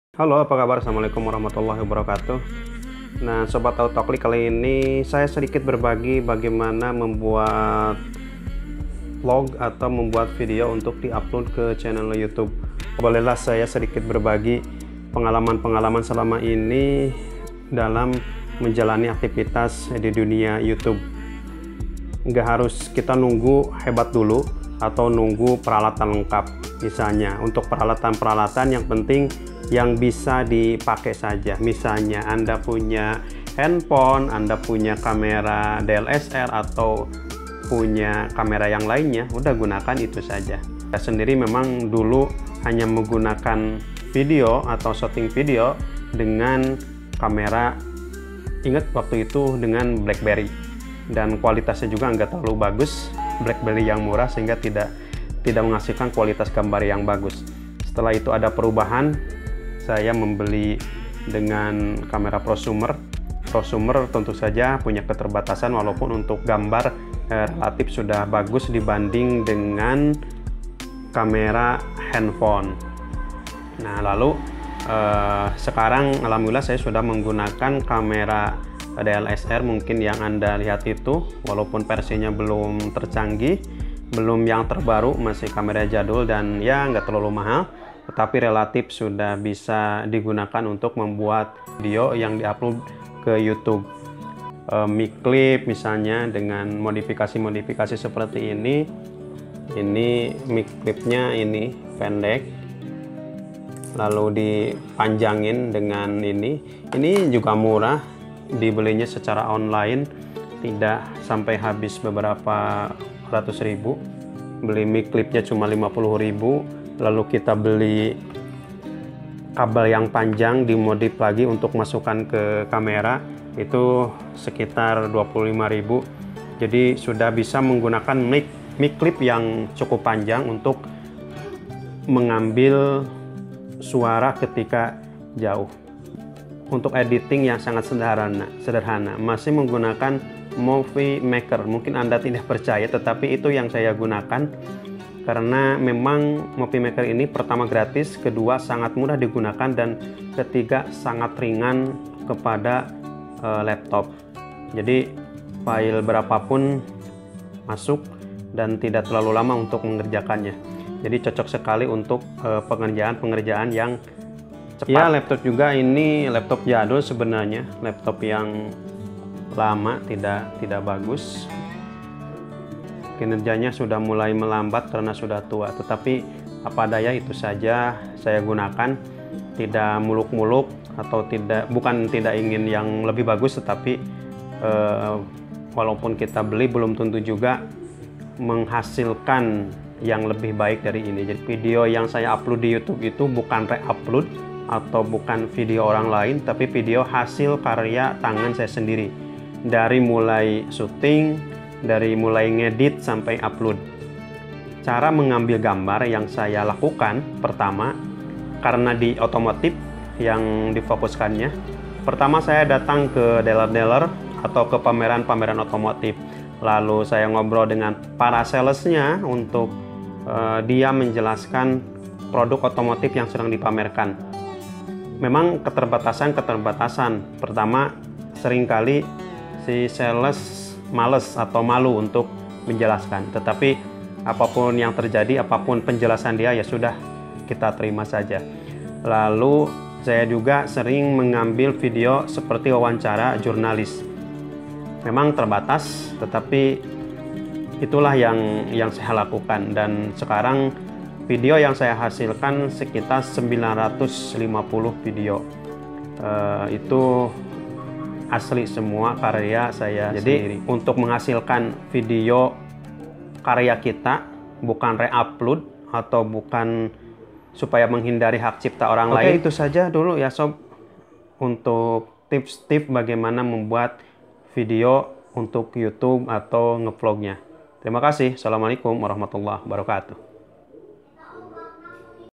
Nge Vlog dengan Alat Sederhana, Begini Caranya
Aktivitas ngevlog atau mmebuat video pribadi sangat menyenangkan. Media soaial kini bis amenayangkan langsung video yang dibuiat dengan jangakauan takj terbatas.

Betikut ini tips membuat video sendiri dengan mudha dan cepat dengan peralatan yang seadanya